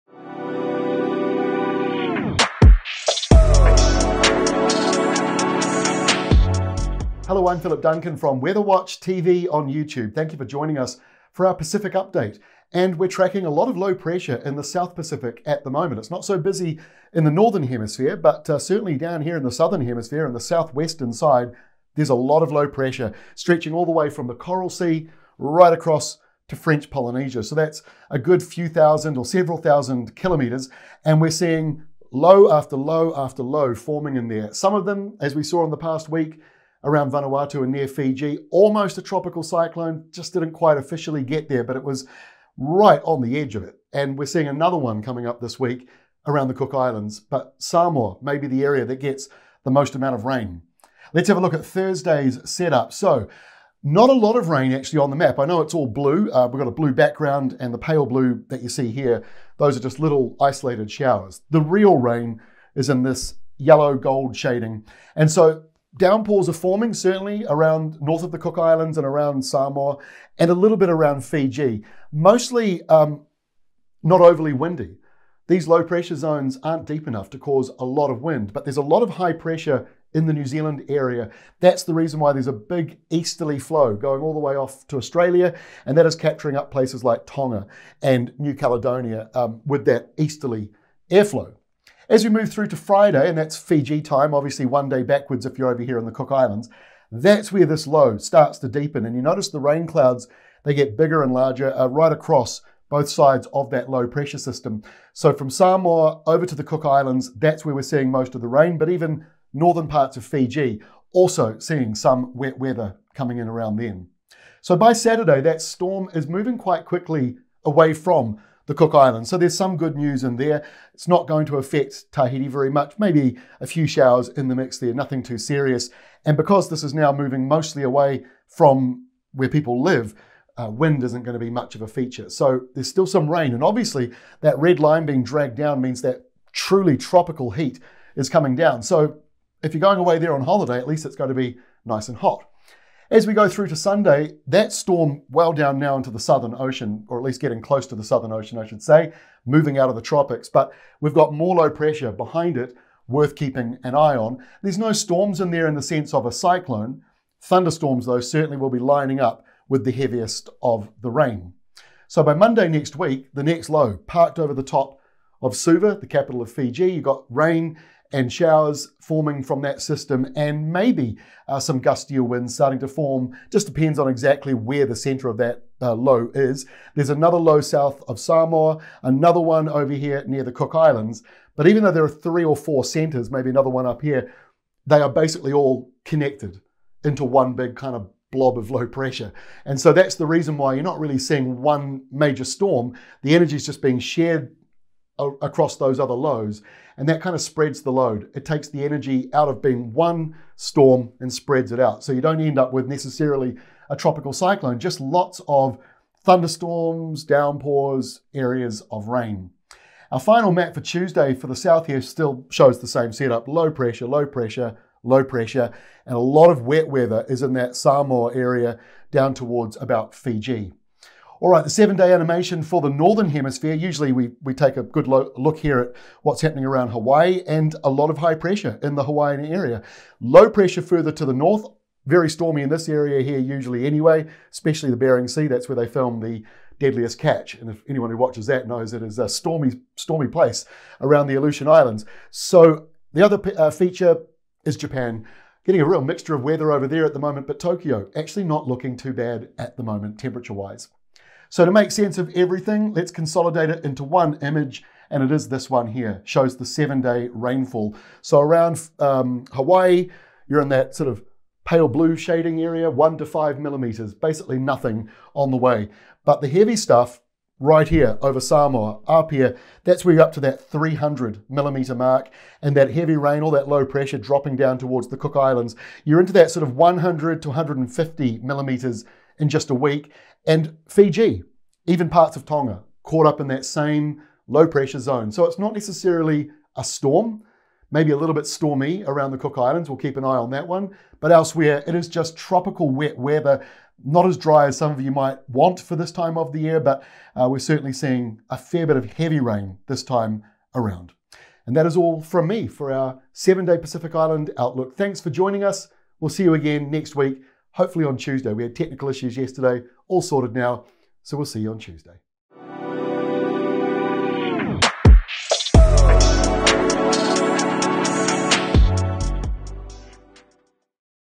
7 Day Pacific Update: Low pressure smothers the South Pacific
It’s one low pressure system after the other in the South Pacific over the coming week with some cyclone potential - but for now it’s a lot of rain coming with places like Samoa potentially getting over 300mm and parts of Fiji, Tonga and the Cook Islands getting plenty of low pressure and areas of rain, thunder and downpours.  It’s hot and humid for everyone in this group, but easterlies between the tropics and New Zealand mean that some nations further south (like Tonga and New Caledonia) may not have non-stop tropical air.

In the Northern Hemisphere it’s mostly settled in Hawaii, stormy around Alaska and Japan has some cold fronts - but actually quite pleasant temperatures in Tokyo.

We make sense of all the wet weather with one single image at the end - the Pacific-wide 7 Day Rainfall Map, exclusive to WeatherWatchTV on YouTube. Thanks for your support of our small business!